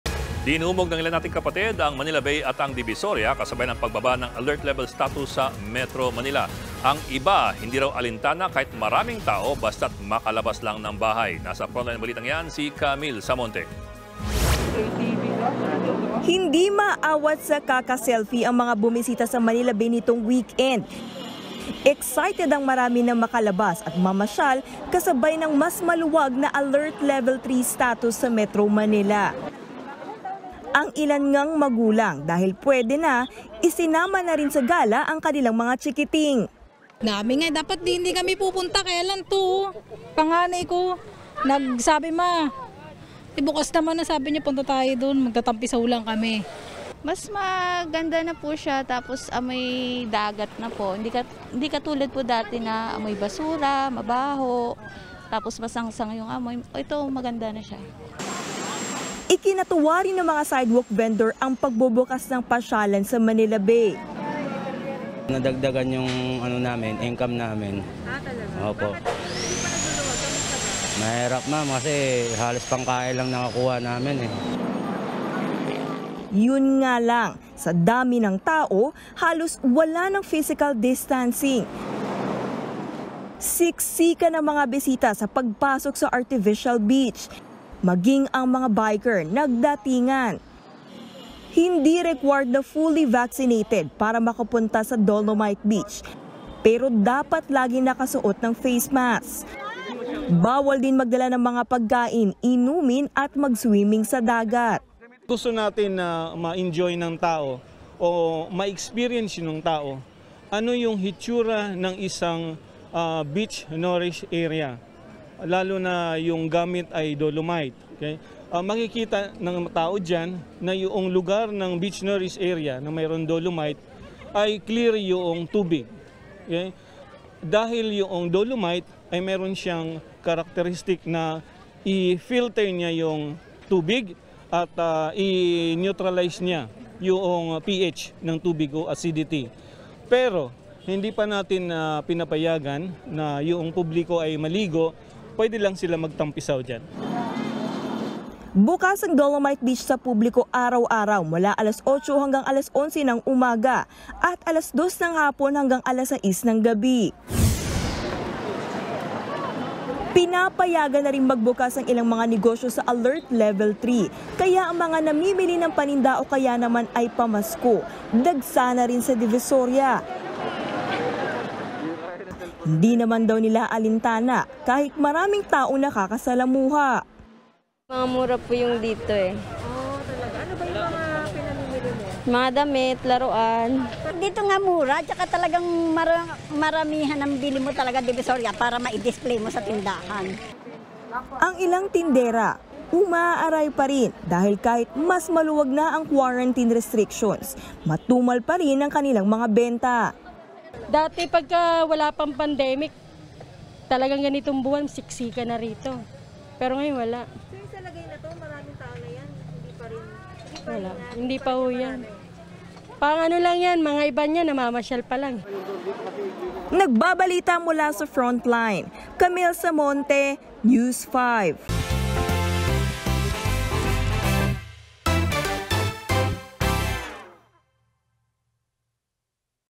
Dolomite beach, dinumog
FrontlineSaUmaga | Dinagsa ng publiko ang Dolomite beach sa Manila Bay kasabay ng mas maluwag na Alert Level status sa Metro Manila. | via Camille Samonte